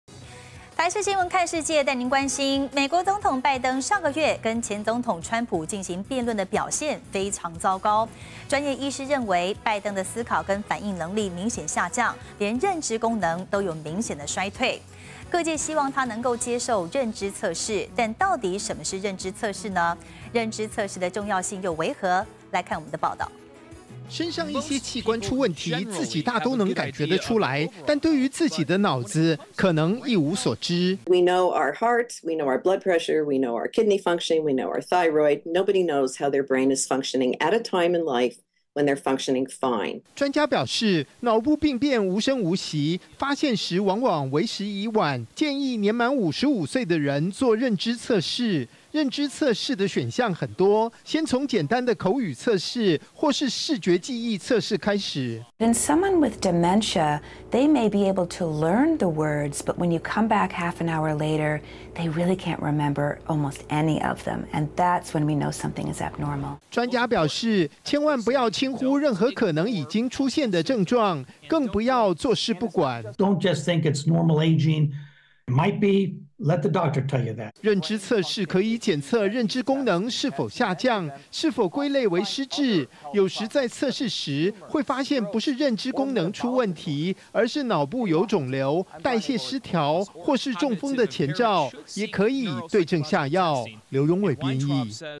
腦病變悄無聲息！ 專家：「滿55歲」建議認知測試
美國總統拜登上個月跟前總統川普進行辯論表現非常糟糕，專業醫師認為拜登的思考和反應能力明顯下降，認知功能明顯衰退，各界希望他接受認知測試，到底什麼是認知測試，認知測試的重要性如何，以下是我們的報導.